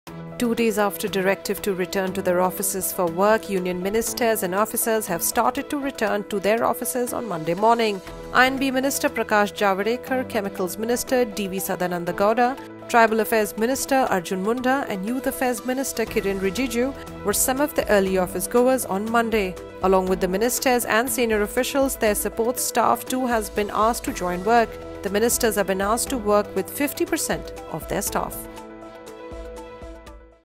Lockdown: Ministers back at offices after govt order
Two days after directive to return to their offices for work, Union Ministers and officers have started to return to their offices on Monday morning. I&B minister Prakash Javadekar, chemicals minister DV Sadananda Gowda, tribal affairs minister Arjun Munda and youth affairs minister Kiren Rijiju were some of the early office-goers on Monday. Along with the ministers and senior officials, their support staff too has been asked to join work. The ministers have been asked to work with 50% of their staff.